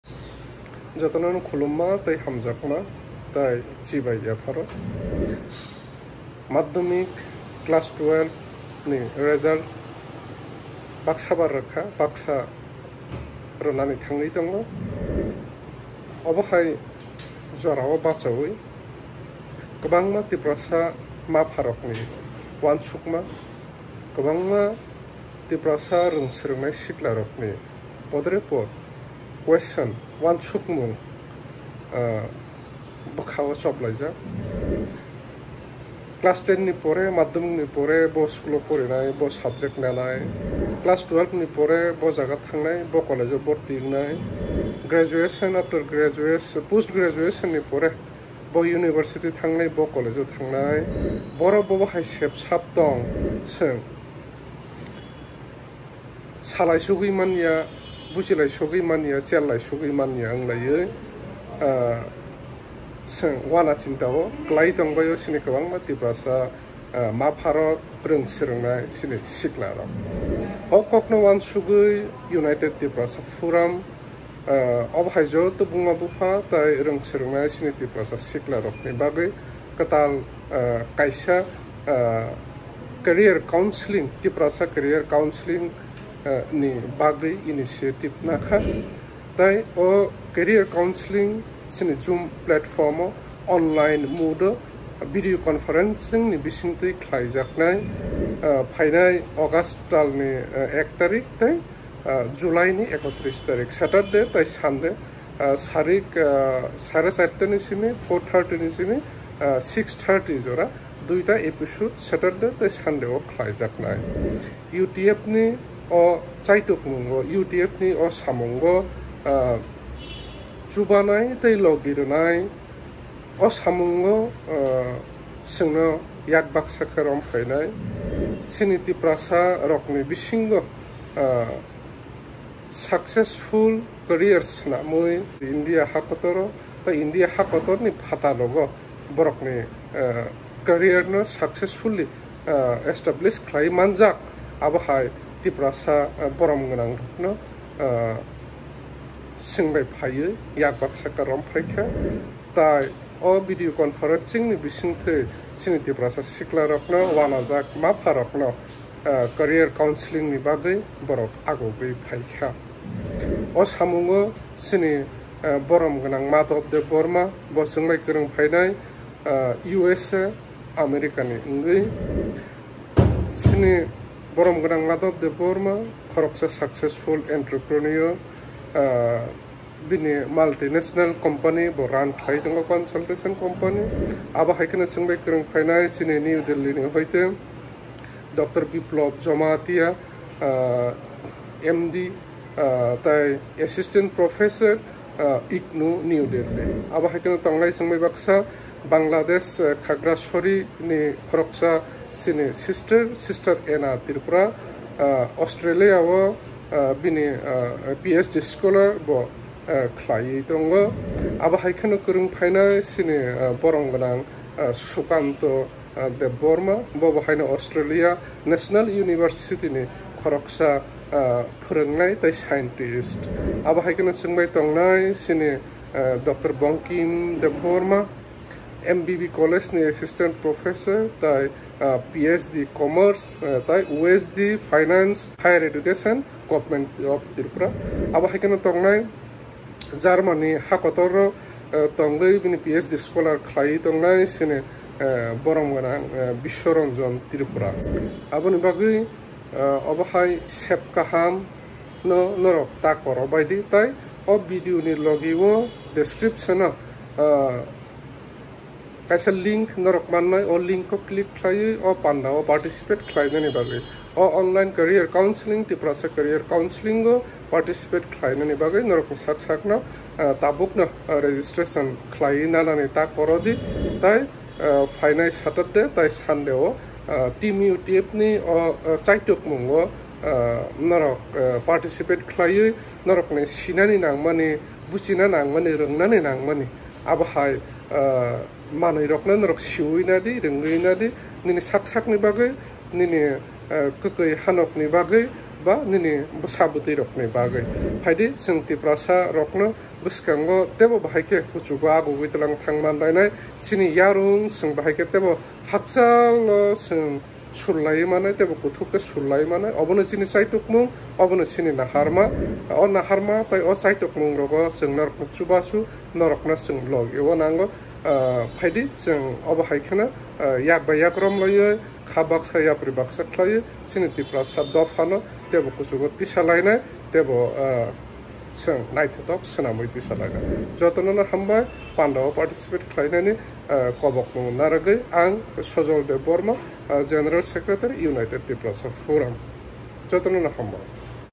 TIPRASA CAREER COUNSELING
Tiprasa siklarokni bagwi kwtal kwtal simwng tei rwngmwngni sebsab tei lamarok phiyokjakthwng hwnwi wansogwi chini oh chaitokmwng. Chini resource persons rokni kisaswk sinimwngrok 1)Madhab Debbarma (B.Tech in Computer Sc | MBA in IIM Bangalore | Enterprise IT & Business Consultant | Owner of HAZELDI CONSULTING INC in USA) ; 2)Dr. Biplab Jamatia (MBBS, MD from AIIMS Delhi | Asst Professor in IGNOU, Delhi) ; 3)Mrs Ena Tripura, Basically From Khagrachhari Bangladesh (Former Alumni of Delhi University | Ph.D Scholar At Flinders University, Australia) ; 4)Sukanta Debbarma (B.E | M.Tech | Scientist Ministry of IT, Govt of India | Semiconductor Process Engineer from Global Foundry, Singapore | Tech Lead, Optofab-ANFF in Australian National University, Canberra, Australia) ; 5)Dr. Bankim Debbarma (Ph.D in Commerce | Asst Professor in MBB College | OSD-Finance, Directorate of Higher Education, Govt of Tripura) ; 6)Mr. Biswaranjan Tripura (Asst Professor of TATA Institute of Social Sciences, Mumbai | Ph.D Scholar in RUB, Germany). Chini panda bwtang : Date - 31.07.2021, Saturday, 4.30PM - 6.30PM, Resource persons - Dr. Biplab Jamatia, Mr. Sukanta Debbarma and Mr. Biswaranjan Tripura || Date - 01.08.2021, Sunday, 4.30PM - 6.30PM, Resource persons - Mr. Madhab Debbarma, Mrs. Ena Tripura and Dr. Bankim Debbarma. Kainwi sessions no ZOOM Platform o wngnai, ZOOM ID tei Password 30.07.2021 salmario jotto 250 registered participants rokno WhatsApp ni Bisingtwi rohorjaknai.

Regards
Team UTF